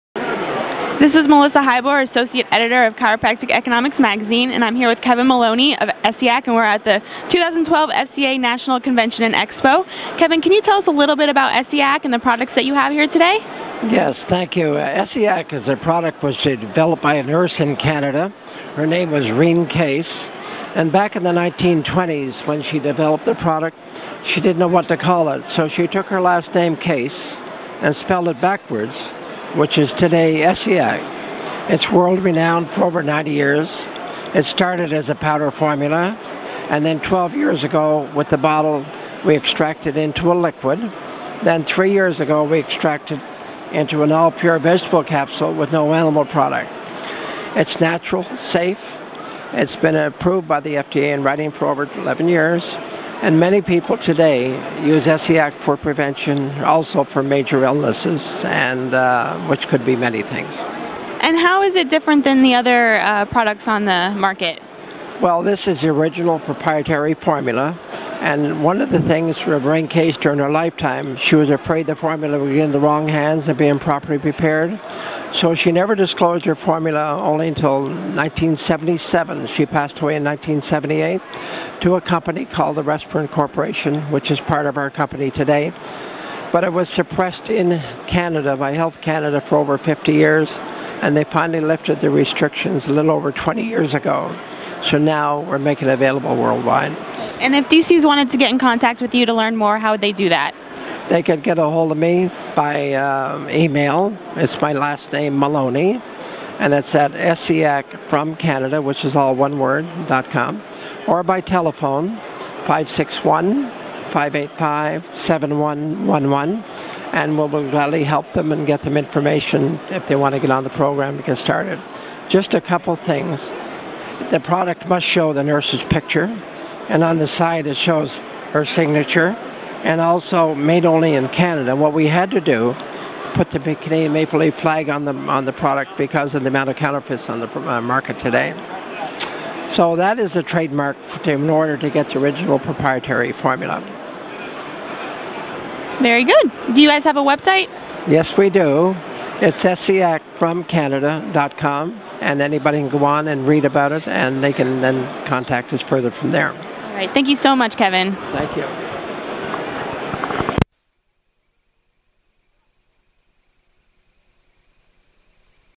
Essiac: FCA 2012
Melissa Heyboer, associate editor of Chiropractic Economics magazine, interviews Kevin Maloney of Essiac at the Florida Chiropractic Association National Convention & Expo 2012.